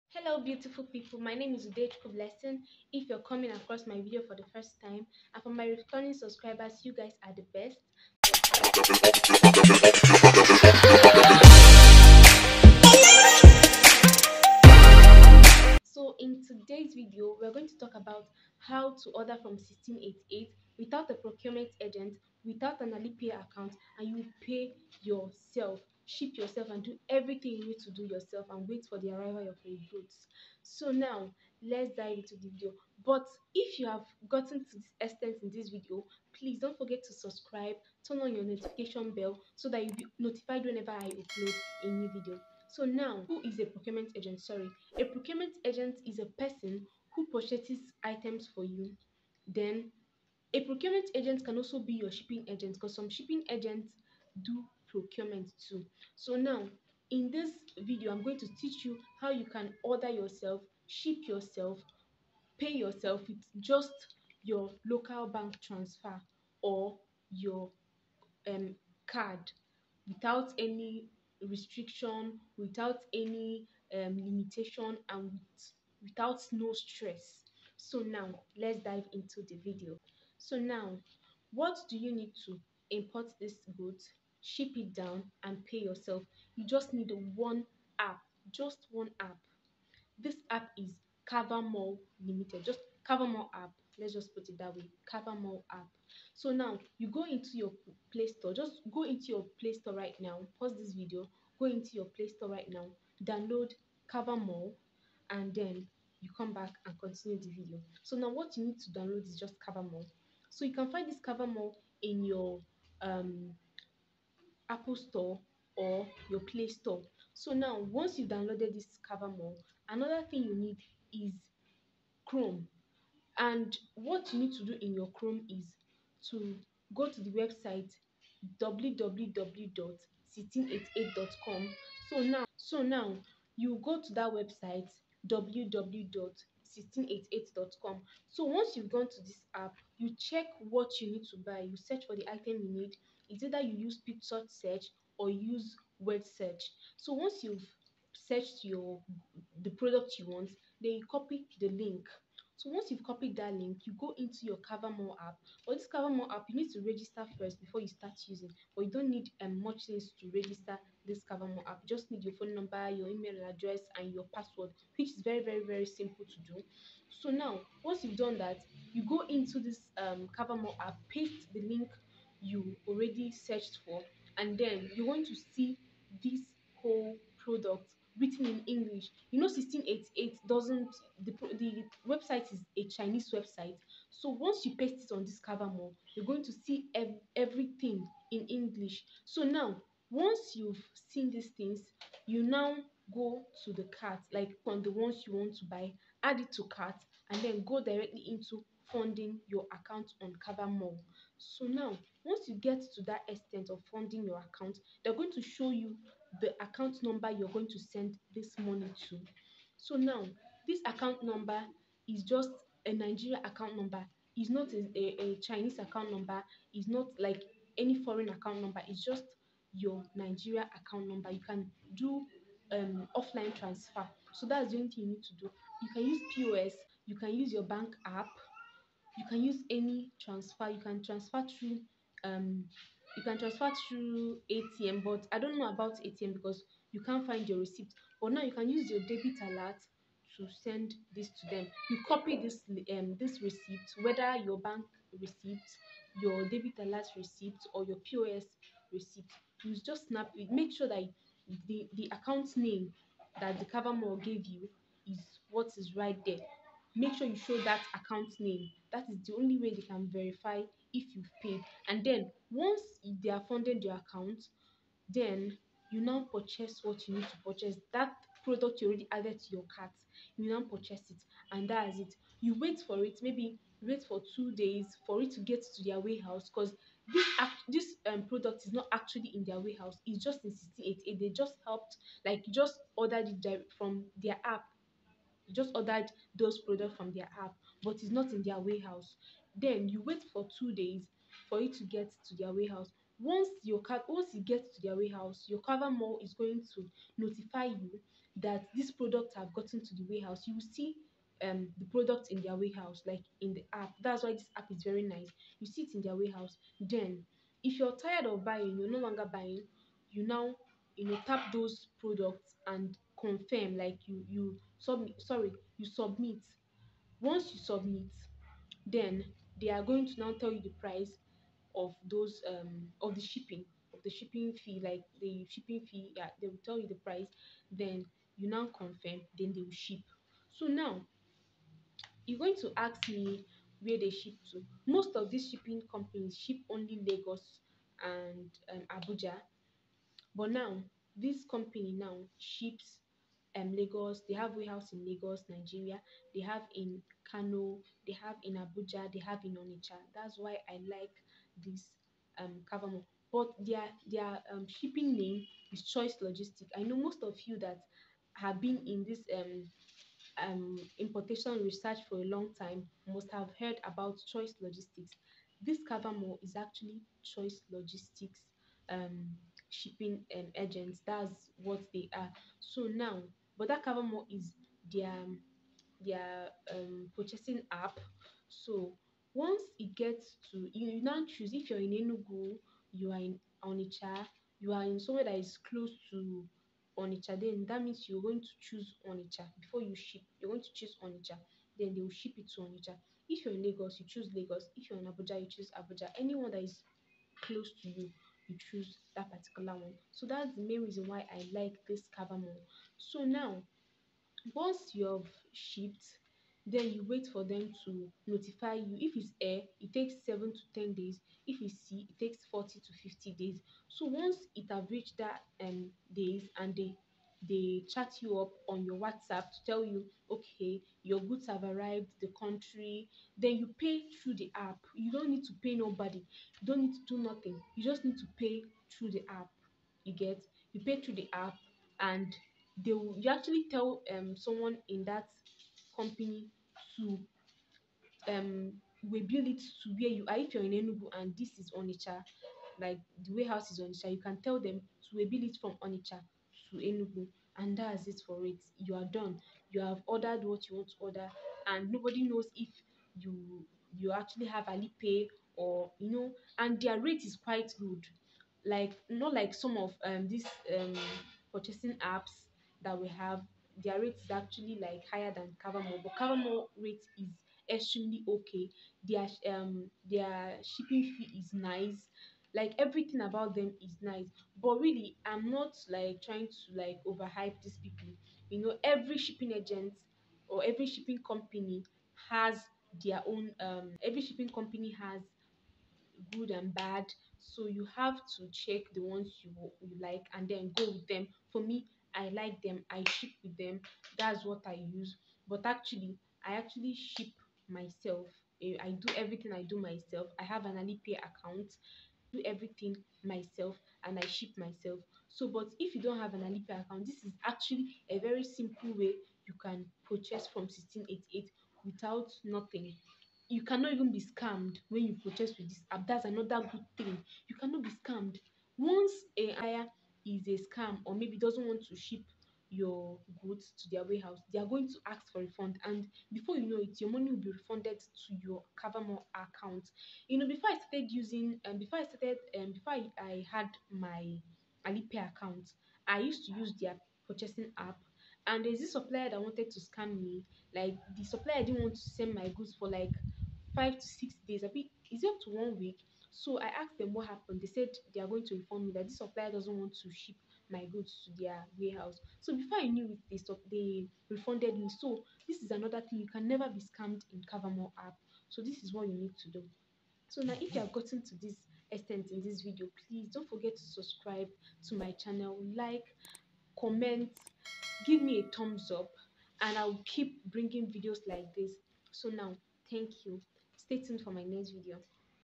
How to order from 1688(china) without an Alipay account/no procurement agent/pay yourself.
Download Cavamall app and sign up, then go to your chrome browser and type www 1688.com search for your preferred item copy the link and go back to cavamall app and paste. Then you go ahead and order.